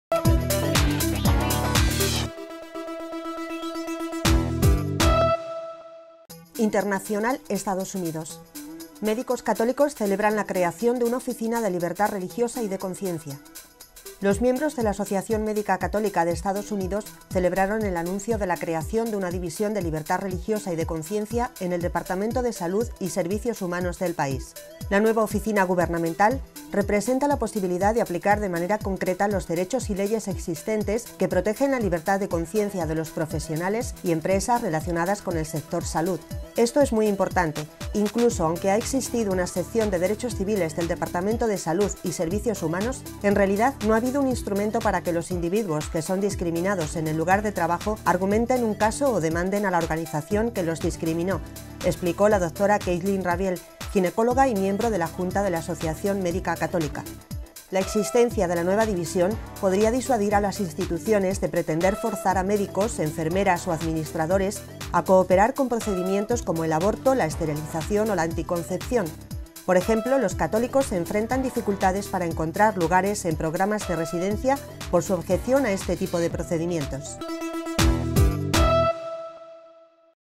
Vídeo Noticia: Médicos católicos celebran creación de oficina de Libertad Religiosa y de Conciencia
Los miembros de la Asociación Médica Católica de Estados Unidos celebraron el anuncio de la creación de una División de Libertad Religiosa y de Conciencia en el Departamento de Salud y Servicios Humanos (HHS) del país.